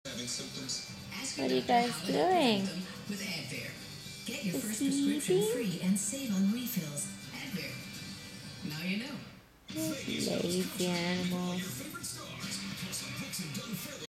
Miniature Horse and Golden Retriever Nap in the Living Room
Mini and Suki are taking a nap together :)